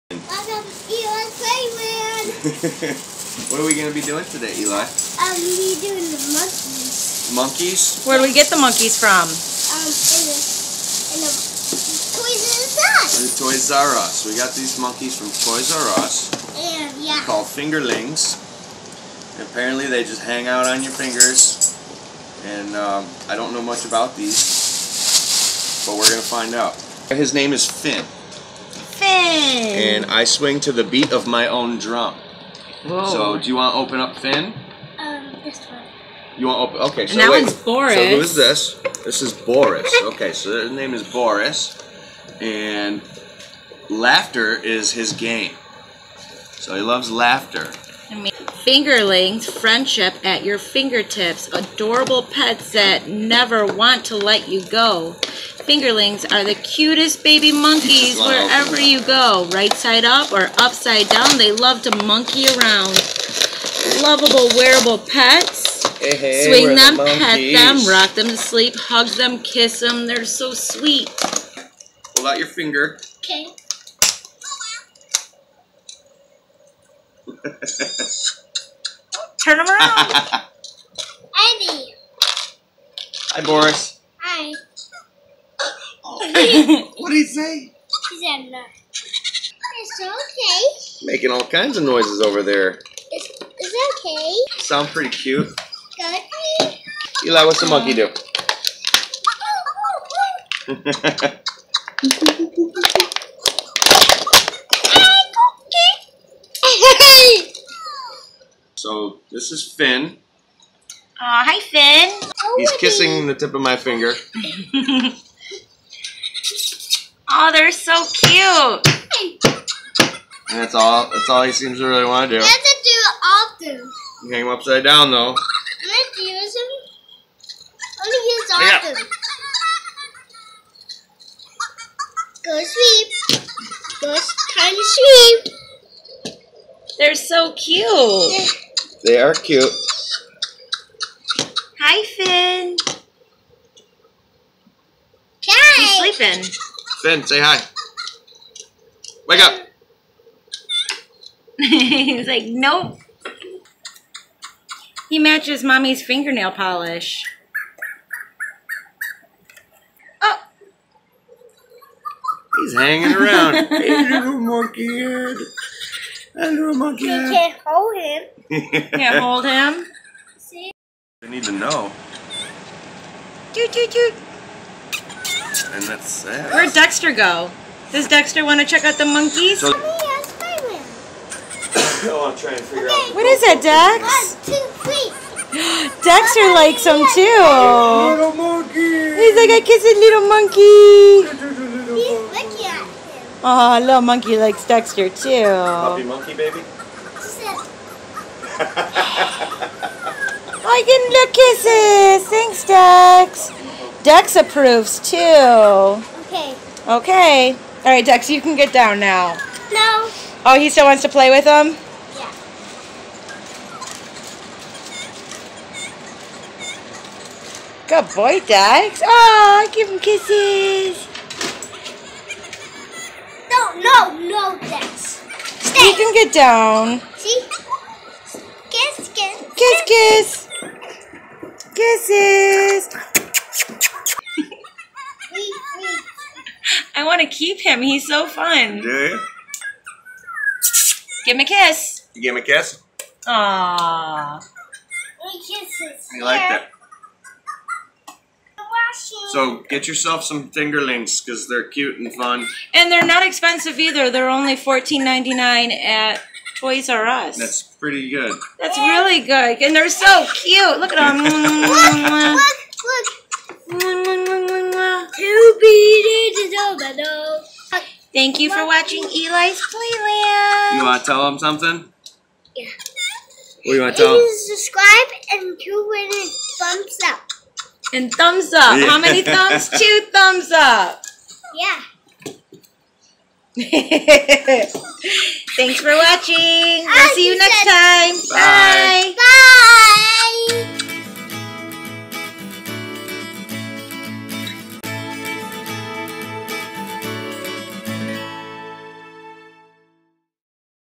FINGERLINGS by WowWee CUTE BABY MONKEYS AT YOUR FINGERTIPS TOY REVIEW/ ELI'S PLAYLAND
Eli, Daddy, Dexter and Mommy review cute new toy. Unboxing FINGERLINGS! Friendship at your fingertips! So cute! So Fun!